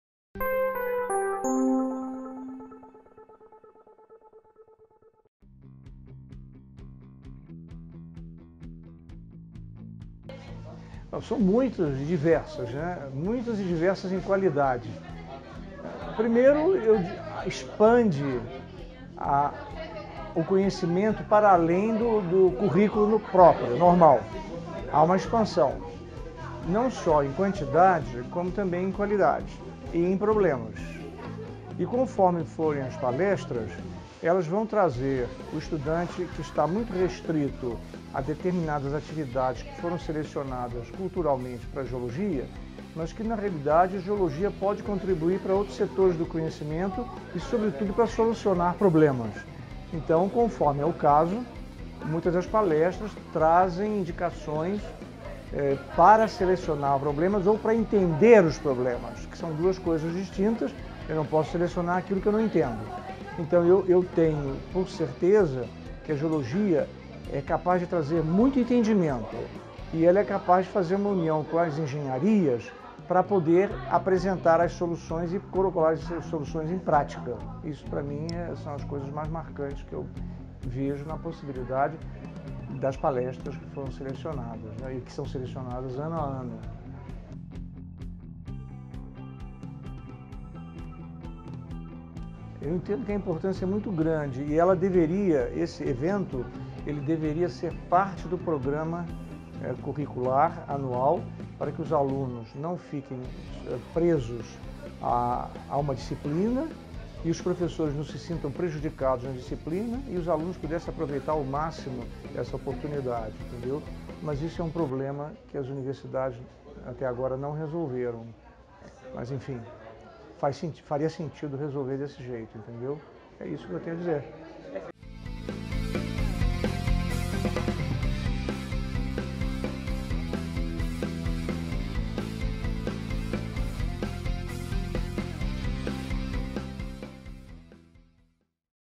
XI Semana da Geologia - Primeiro dia (Entrevista com Paulo Martins)
Professor Doutor Paulo Martins sobre a XI Semana da Geologia.
Segunda-feira, dia 21/08/2017. UFMG.